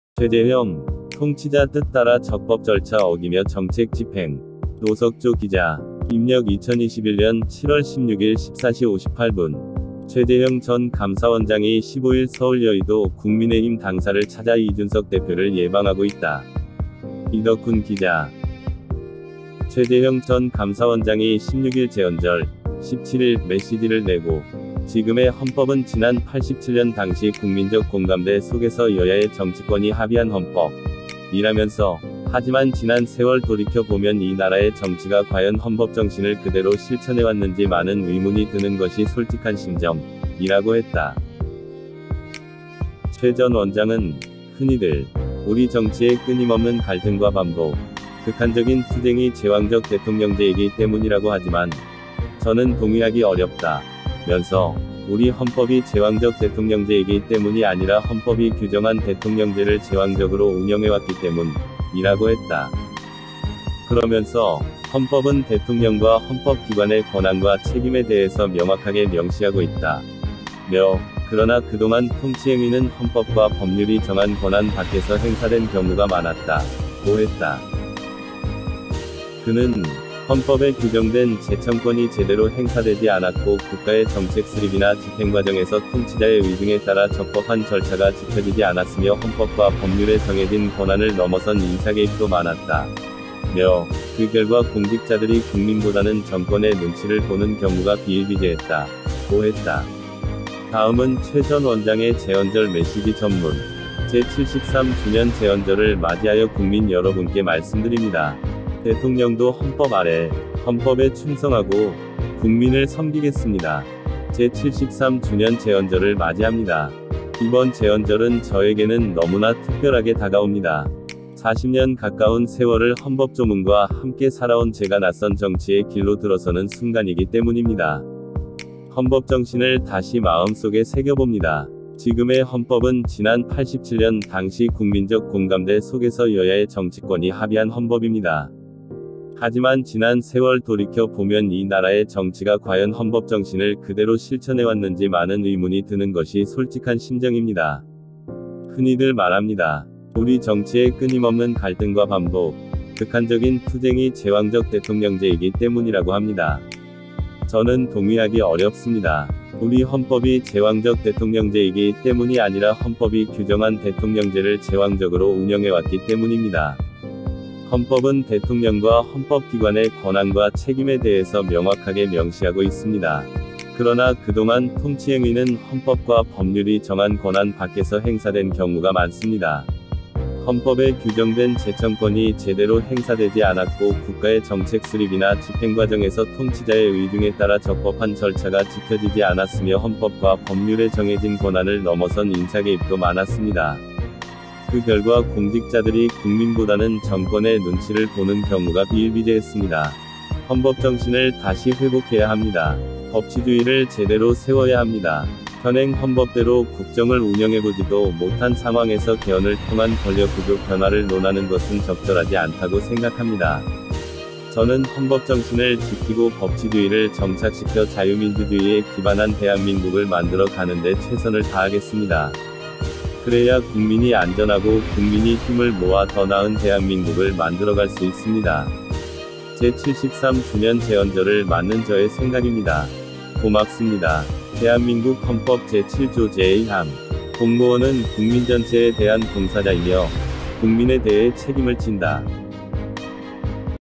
최재형 “통치자 뜻 따라 적법 절차 어기며 정책 집행”
자발적인 시청료 우체국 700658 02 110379

연속듣기
박근혜 대통령_연설문,탄핵인사이드아웃,지나간탄핵다가올탄핵,진실,기획된탄핵조작된민심,미국인박사가파헤친박근혜탄핵의진실,어둠과위선의기록